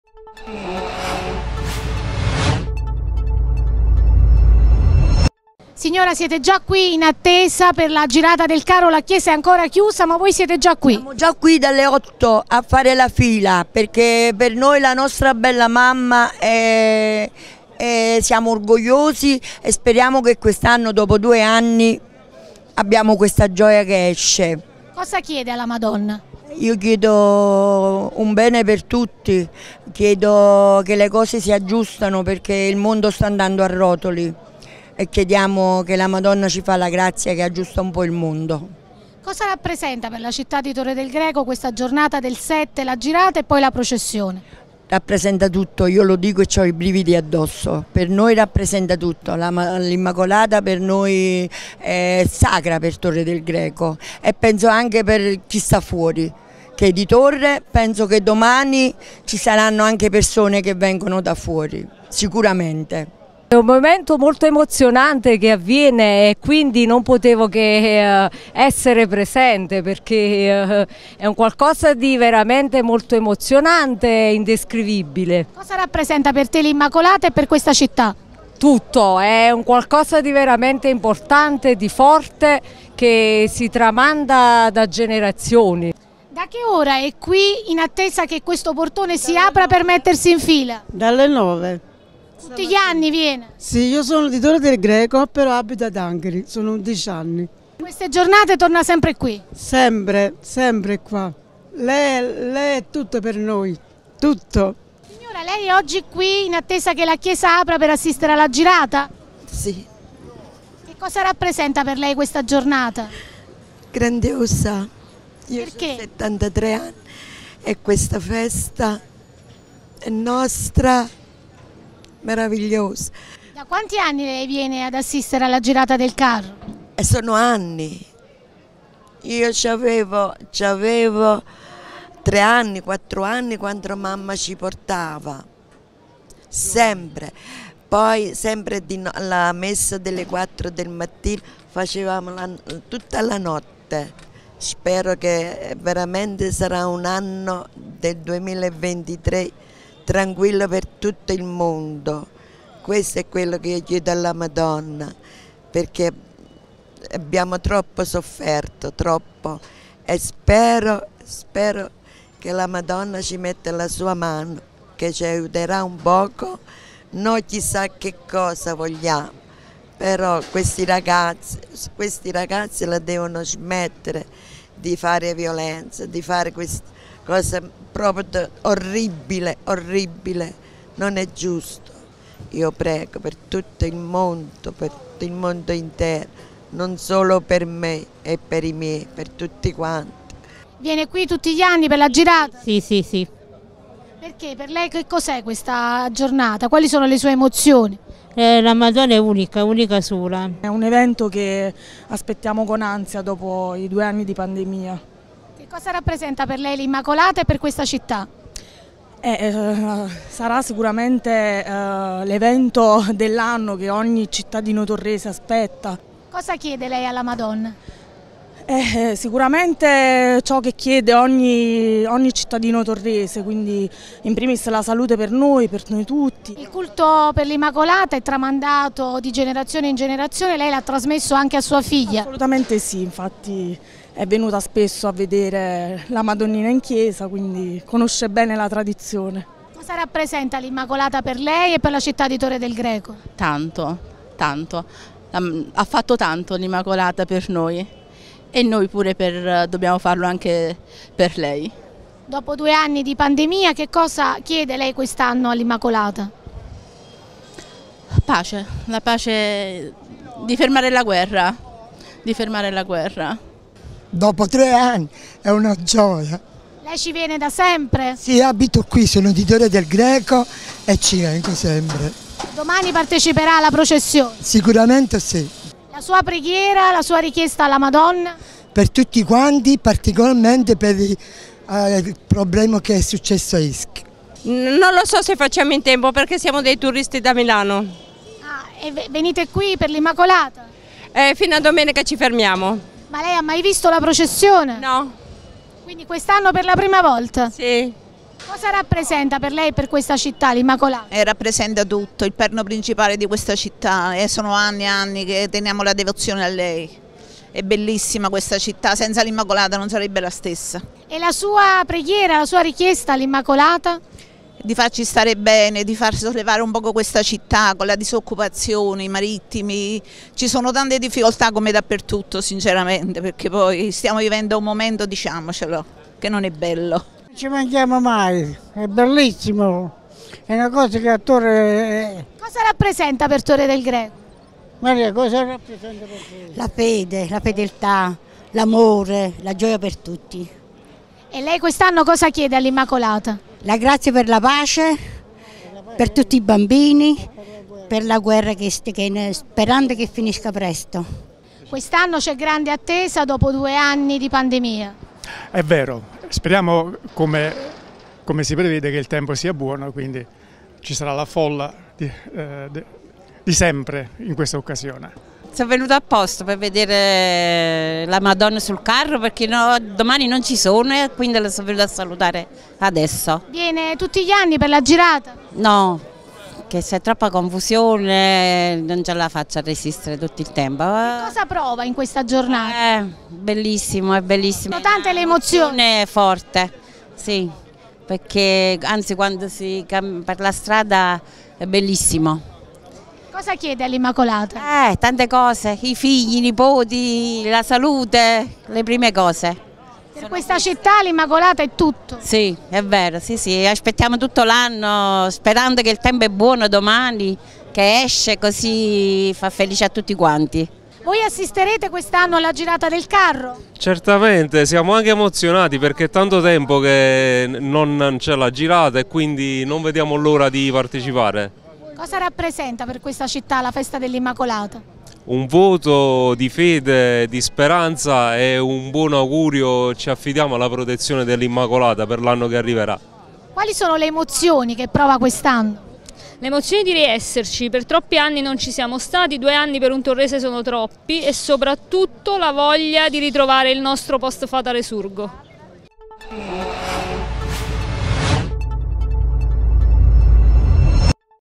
La girata del carro dell'Immacolata, le emozioni dalla piazza
Fedeli in fila dalle 8 di questa mattina in attesa all'esterno della Basilica Pontificia di Santa Croce per assistere dopo 2 anni alla girata del carro dell'Immacolata.

"L'Immacolata per noi è tutto.

E' un momento emozionante, indescrivibile".

Un appuntamento imperdibile per i cittadini di Torre del Greco.

I fedeli affidano all'Immacolata le loro speranze e le loro preghiere, per la salute, la famiglia e la pace nel mondo.